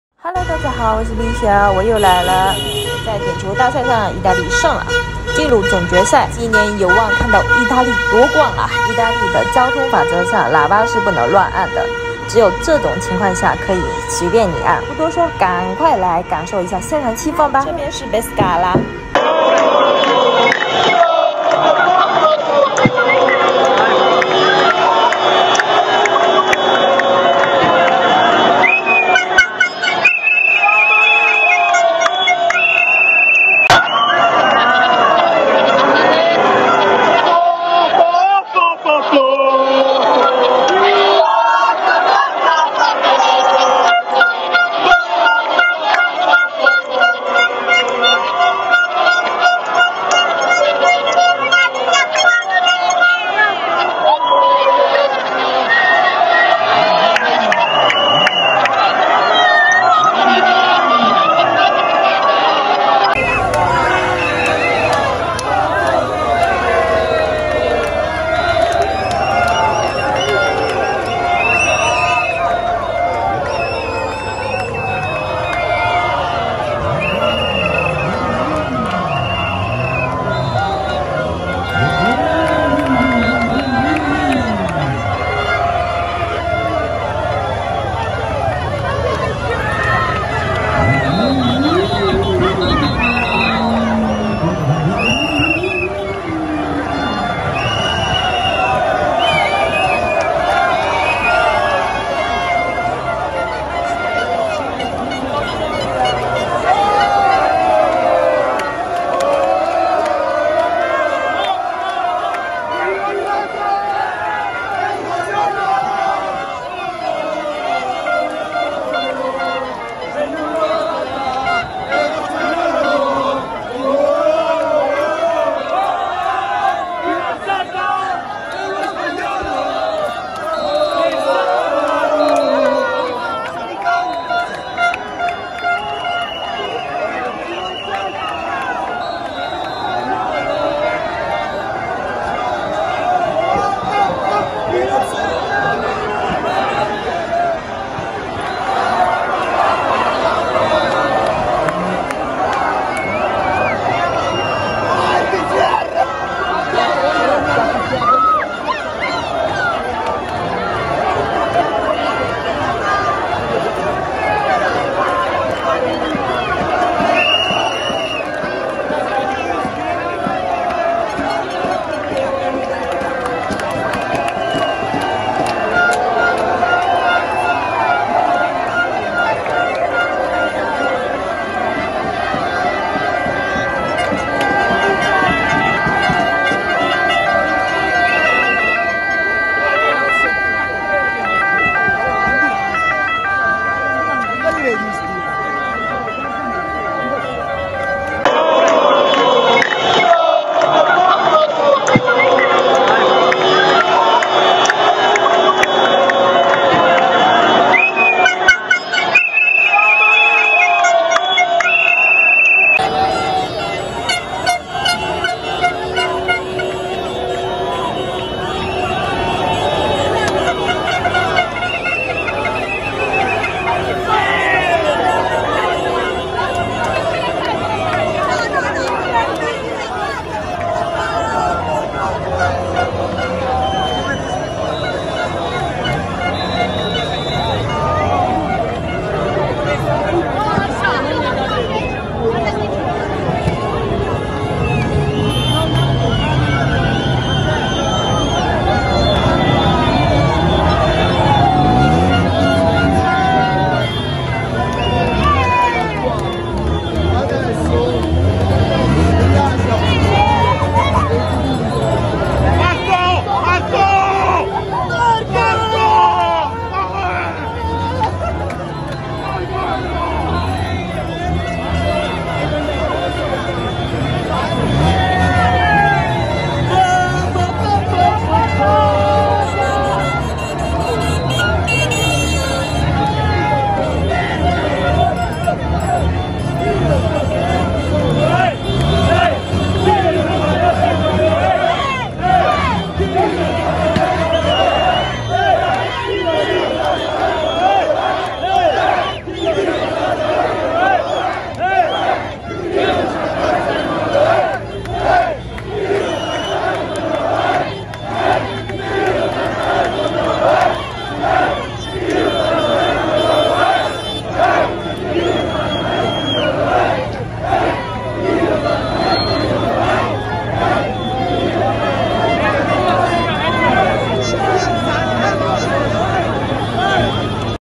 Europei 2021|Italia in Finale｜Pescara意大利进入总决赛啦！感受海滨人民的欢呼
和意呆们一起感受胜利的喜悦！